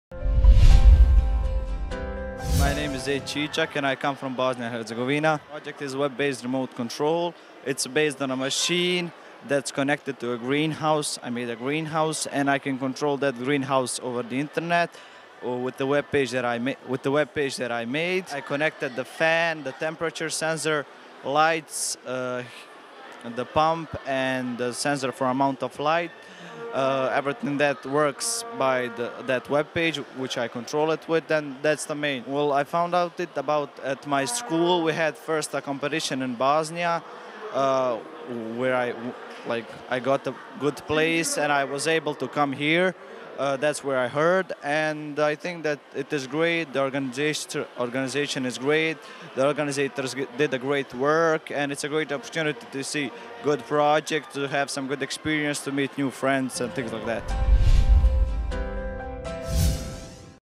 Infomatrix 2014 World Finals // Web Based Remote Control by Zejd Cicak
Project Name : Web Based Remote Control
Category : Hardware Control
Country : Bosnia And Herzegovina
Made by : Zejd Cicak